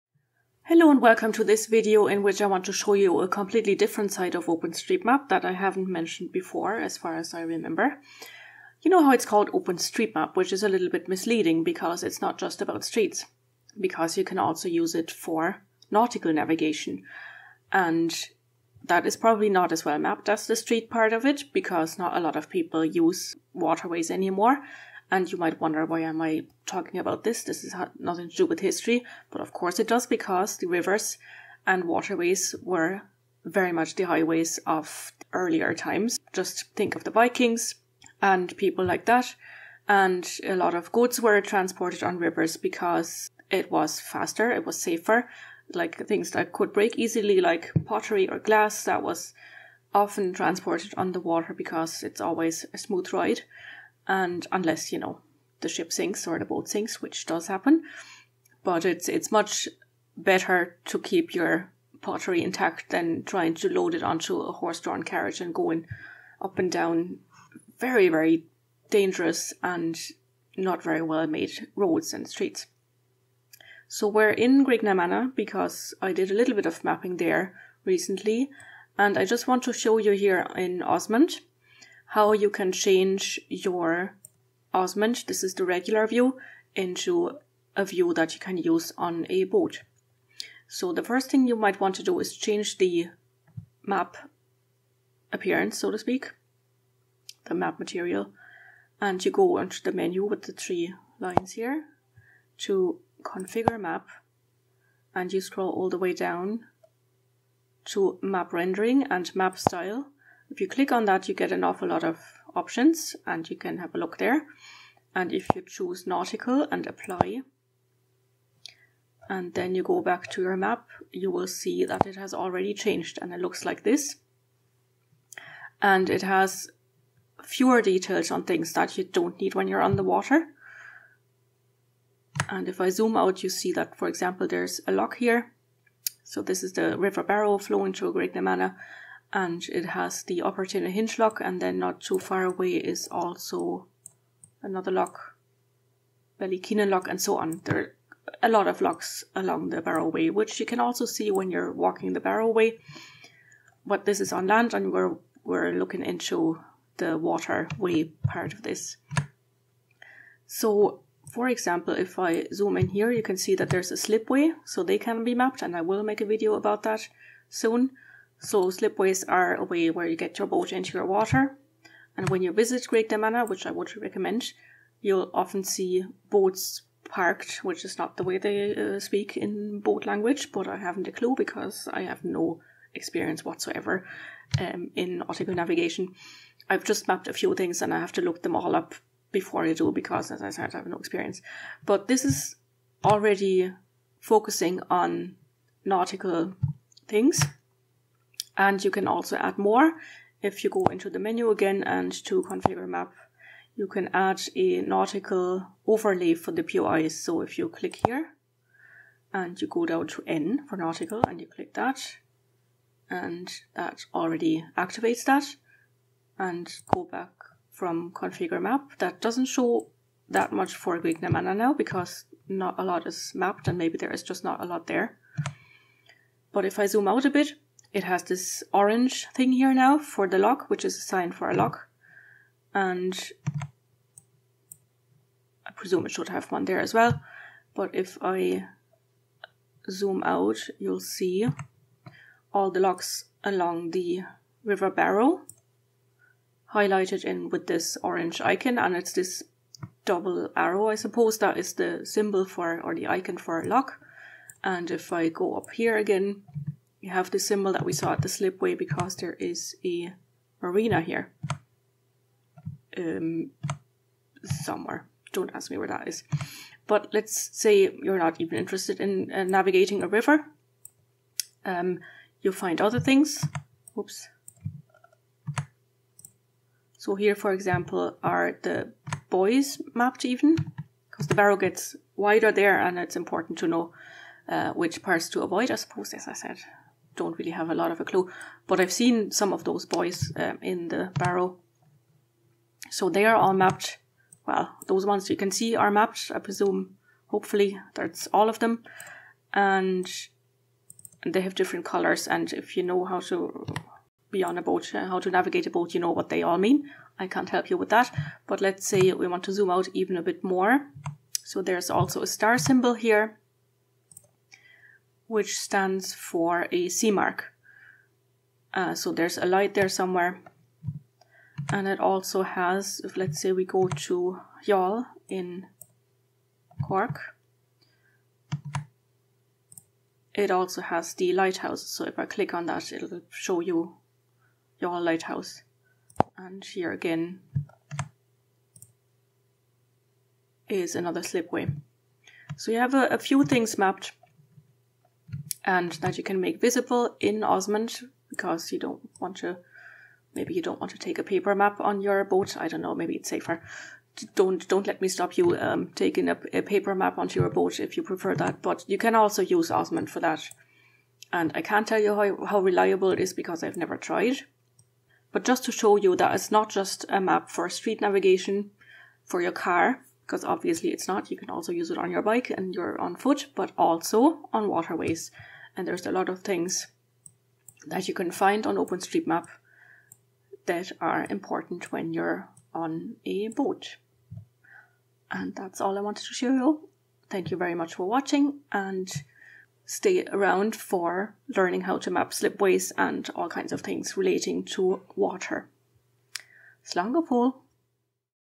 Nautical Map on OSMAnd
In case you want to use OSMAnd on a boat, this is how you change your map to focus on nautical things like locks, sea marks, light houses etc.

00:00 Intro
00:27 What's the historical connection?
01:39 Changing the background map to nautical
03:18 Adding nautical POIs to the map display
03:48 Some examples of nautical POIs